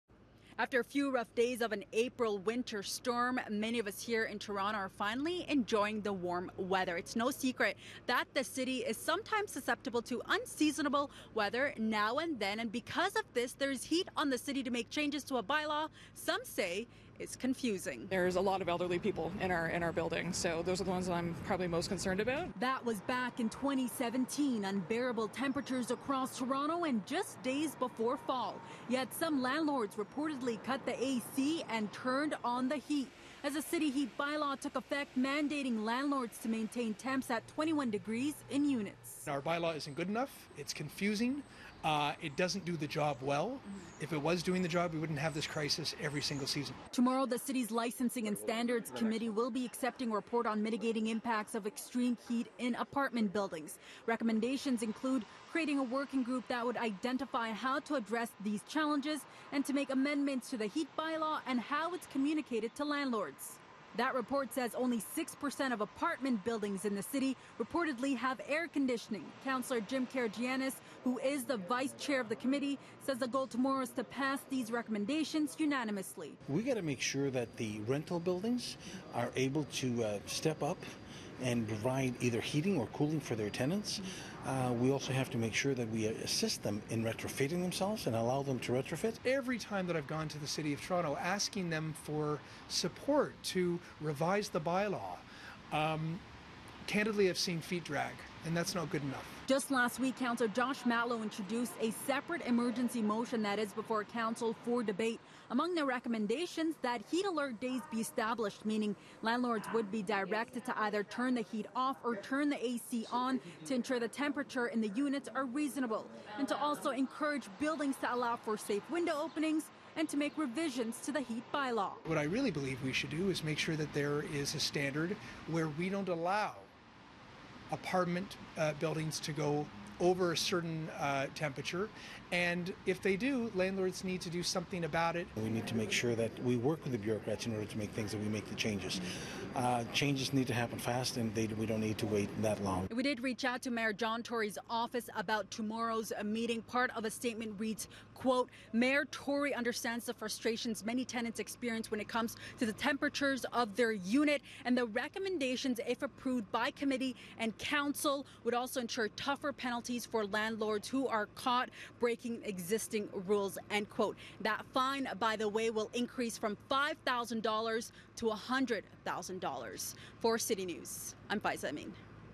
All Toronto apartment buildings should provide relief from extreme heat, report finds
A new staff report recommends that all apartment buildings in Toronto provide relief from extreme heat, with air conditioning and other measures.